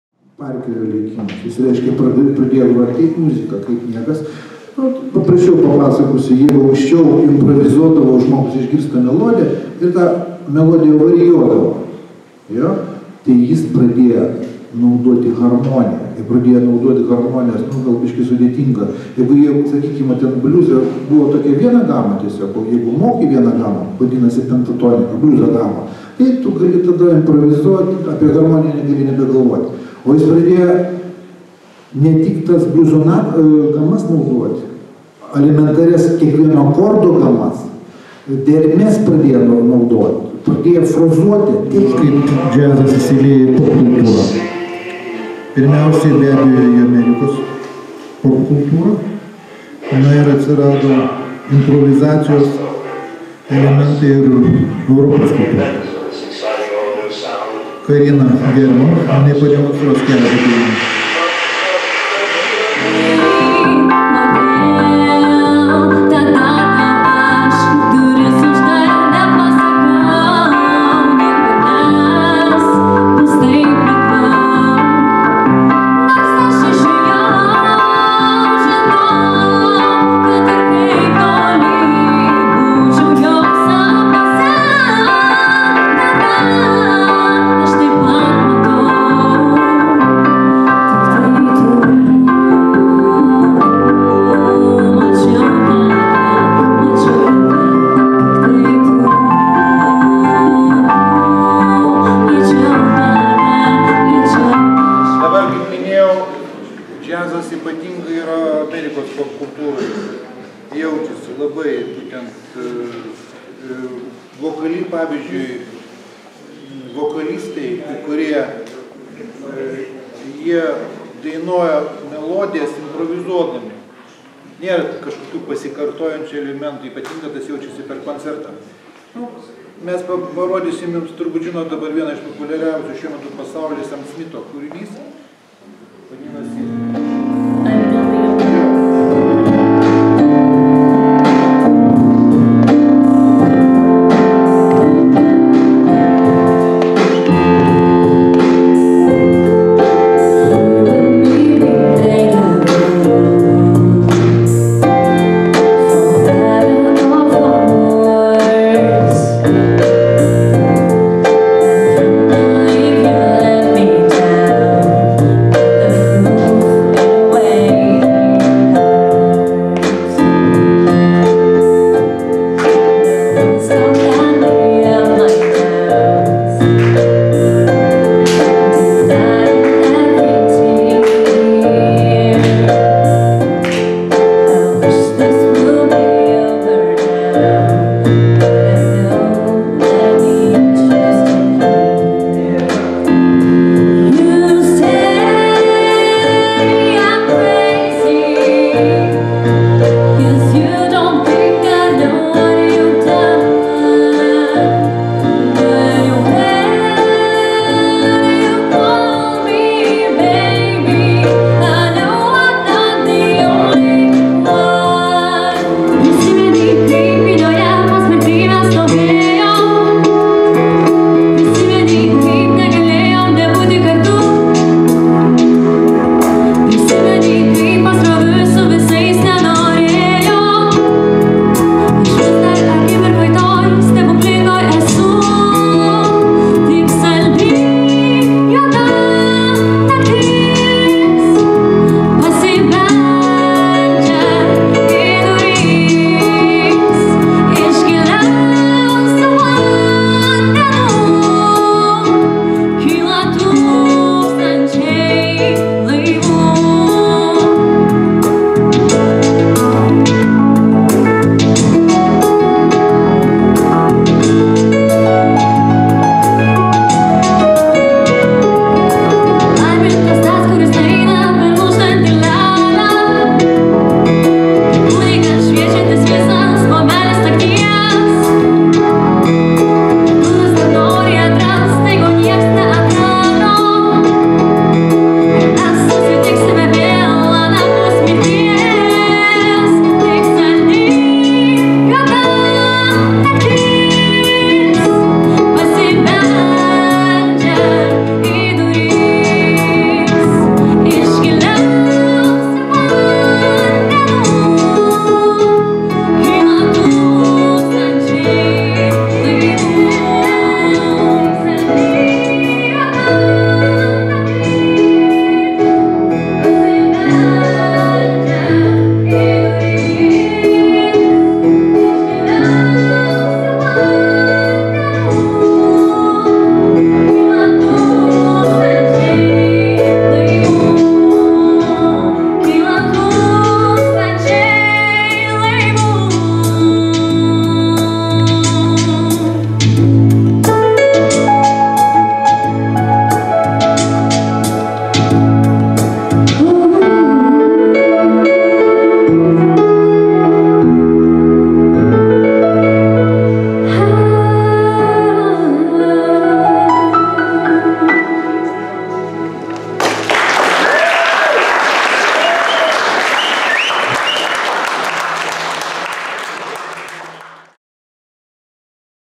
Paskaita „Džiazo muzikos ištakos ir džiazo invazija į popkultūrą"
Paskaitoje dalyvavo muzikos vadovas Vytautas Pilibavičius ir žinoma vokalistė Karina German.